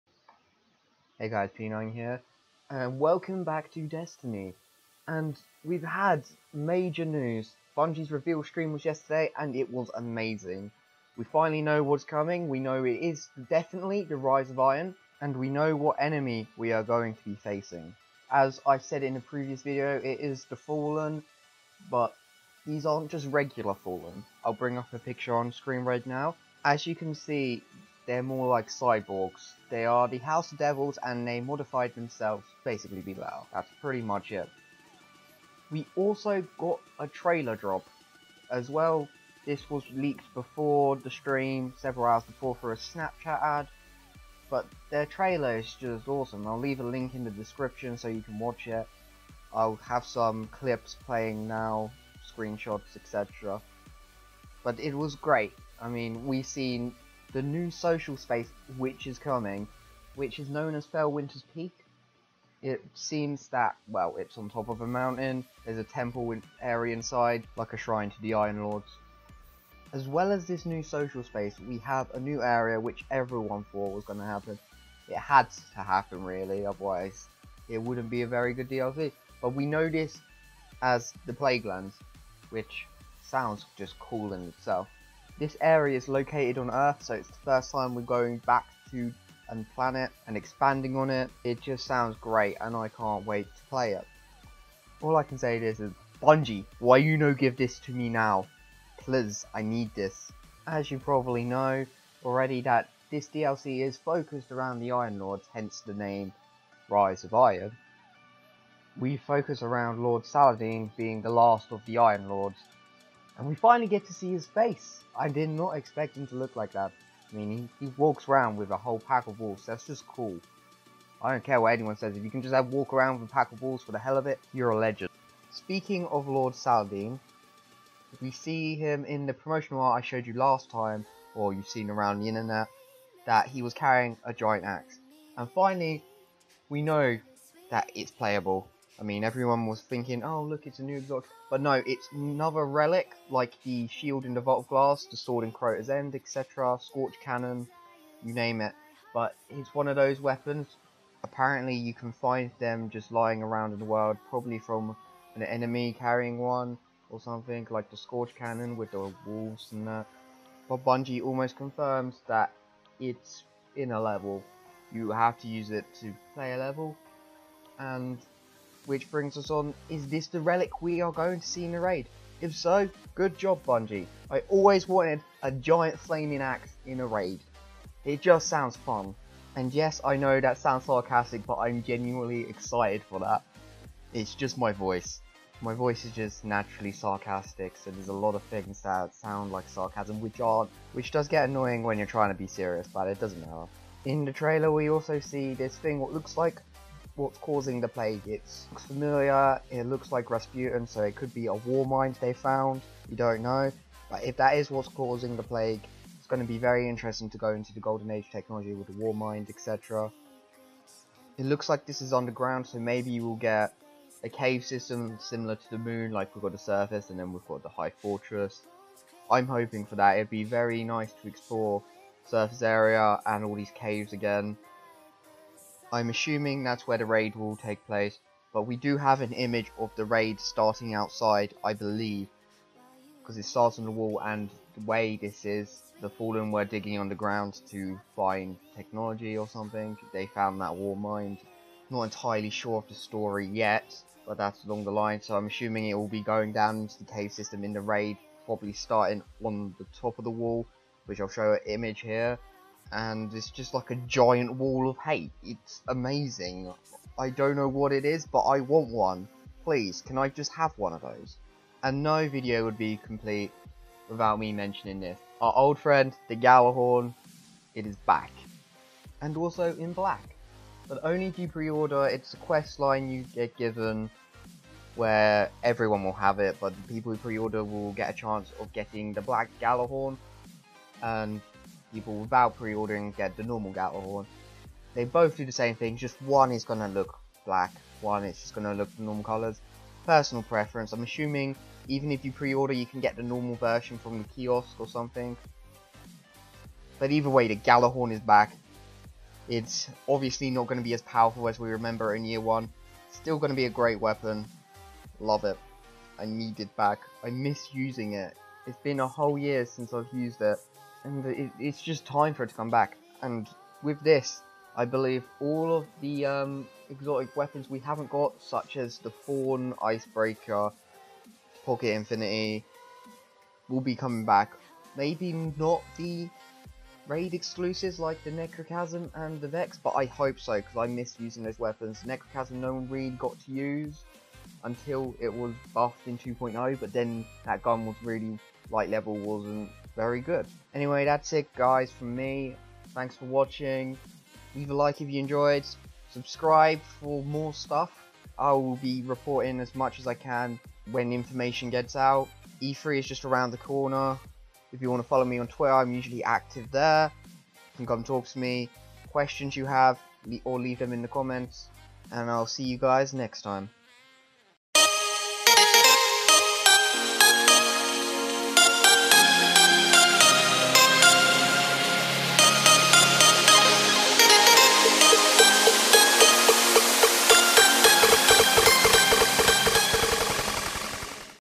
Destiny rise of iron reveal round up!
The new destiny DLC has been announced! Rise of iron sees the story of the iron lords.
Confirmed current day, fallen splicers are afte us, they look kinda creepy! into the mountains we go, too felwinters peak!

lord saladin has a pack of wolves
All the new stuff is coming 9/20/2016
IRON GHORN IS A THING!

My Iron Is rising ( ͡° ͜ʖ ͡°)